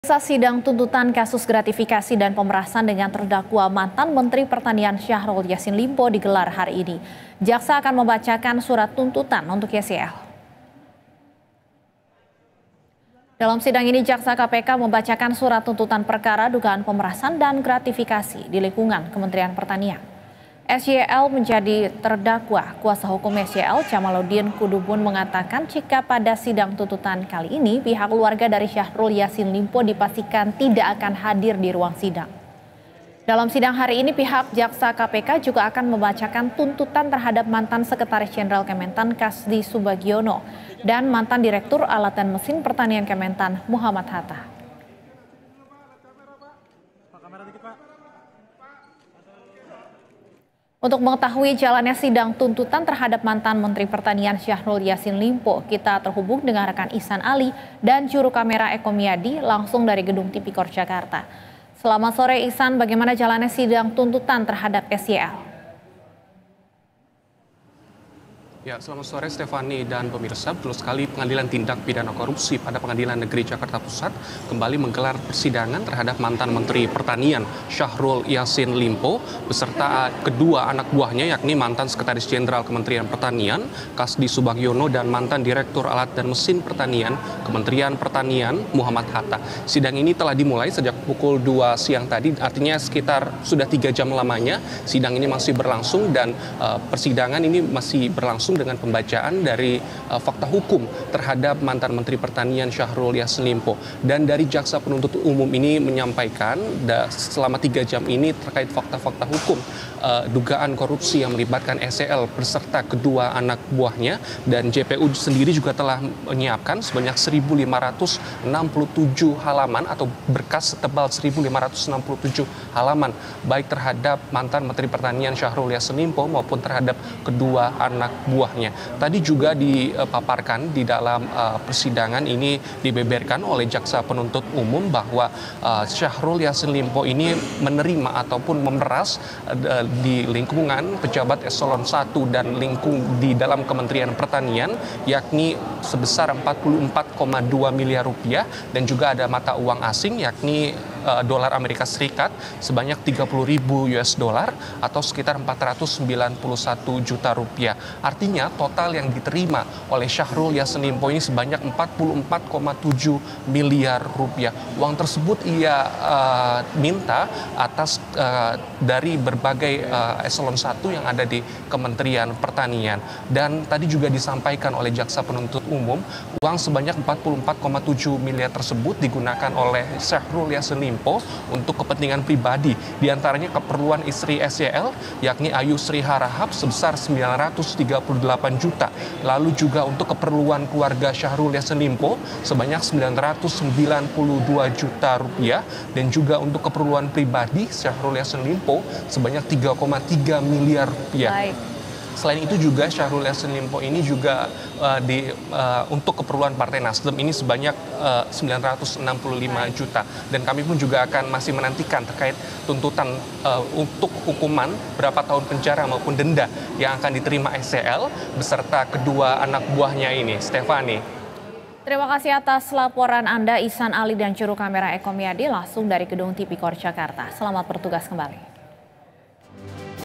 Syahrul Yasin Limpo Hadapi Sidang Tuntutan Kasus Pemerasan | Beritasatu
Sidang tuntutan kasus gratifikasi dan pemerasan dengan terdakwa mantan Menteri Pertanian Syahrul Yasin Limpo digelar. Jaksa akan membacakan surat tuntutan untuk SYL.